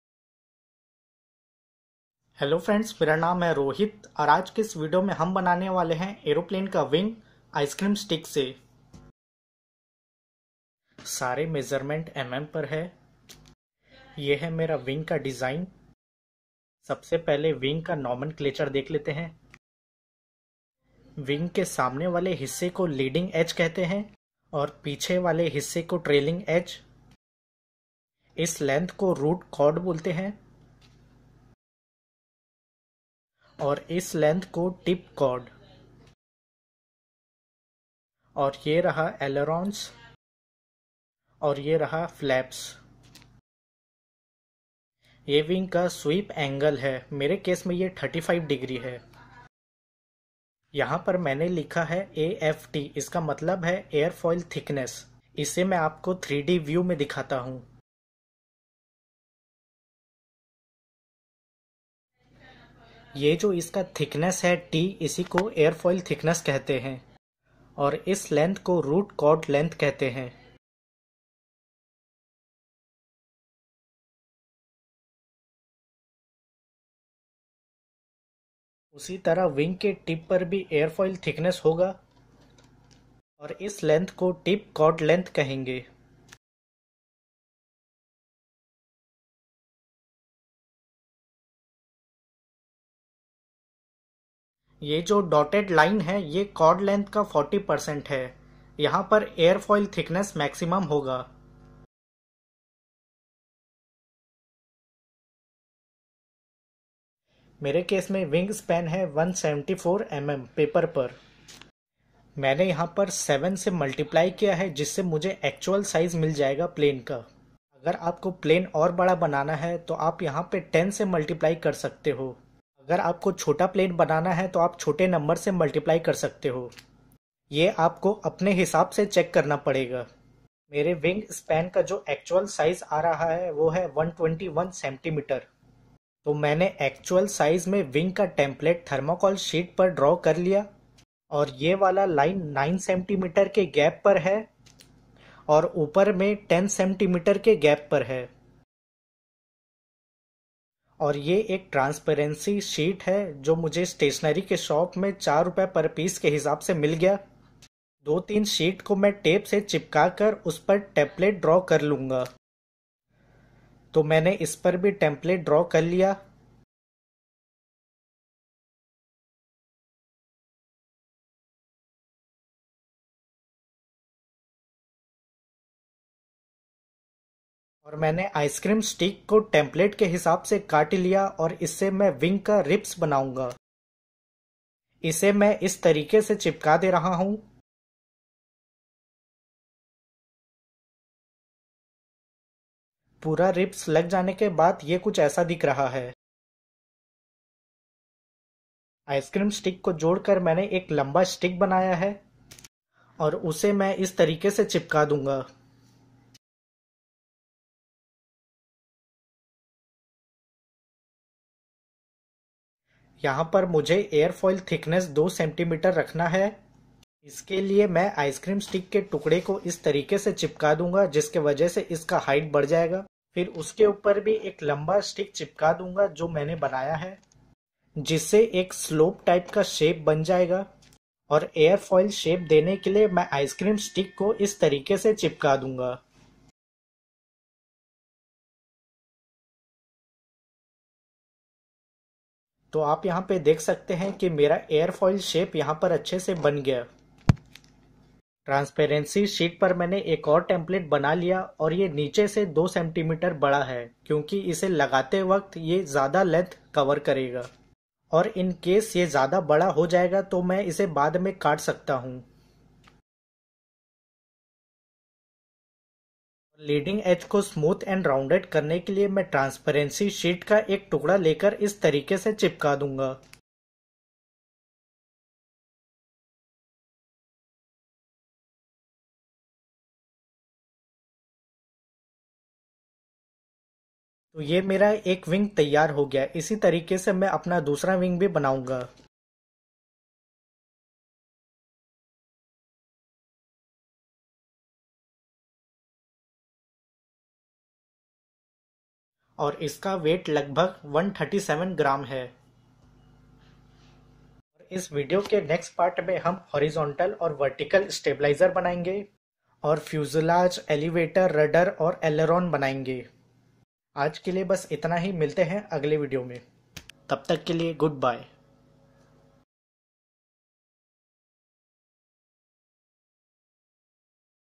RC Plane Wing Build
Hello Friends today i am going to show you how you can design and build your rc plane wing at home.

Background and Outro  -  Photo by Luke Chesser on Unsplash